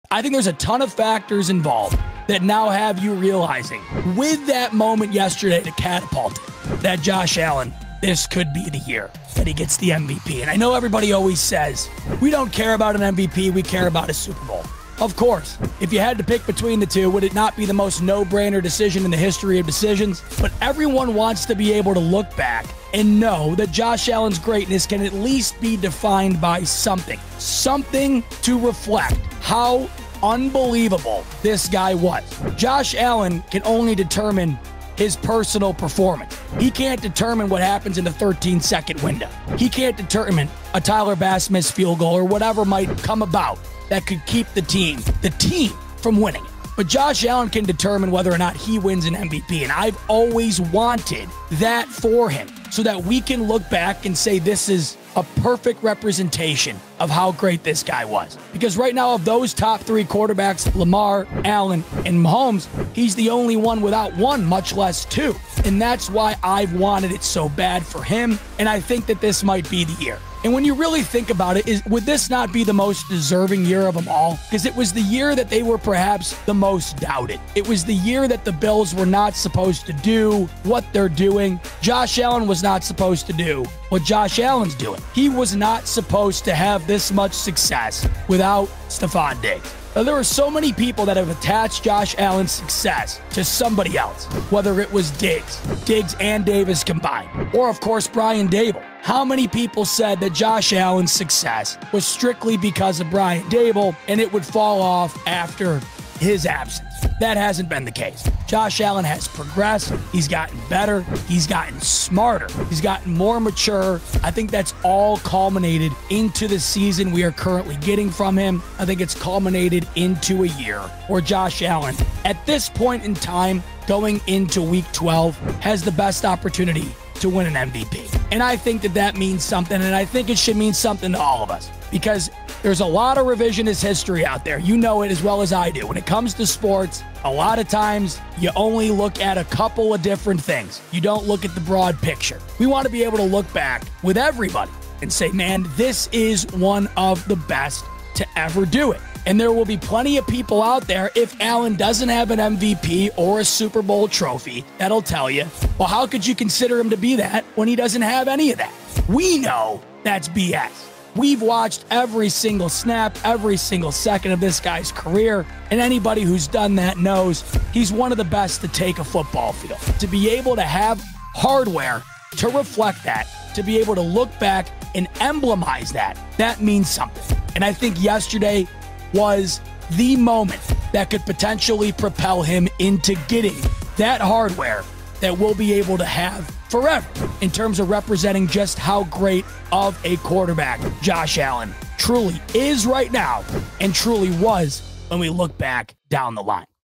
Why Josh Allen Winning MVP Matters
Z-Bot explains that while an MVP trophy for Josh Allen would pale in comparison to a Super Bowl trophy... him winning it still matters.